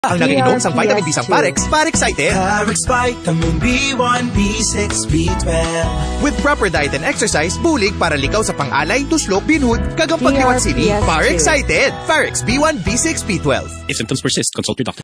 Pharex Vitamin B1 + B6 + B12 Radio Ad 2023 15s (Hiligaynon Version)
Ito po ang 15-seconder version ng 2023 Radio Ad ng Pharex Vitamin B1 + B6 + B12 - Pharexited! (Hiligaynon Version)

DRPS Two 3512: Pharex Vitamin B1 + B6 + B12 Radio Ad 2023 15s (Hiligaynon Version)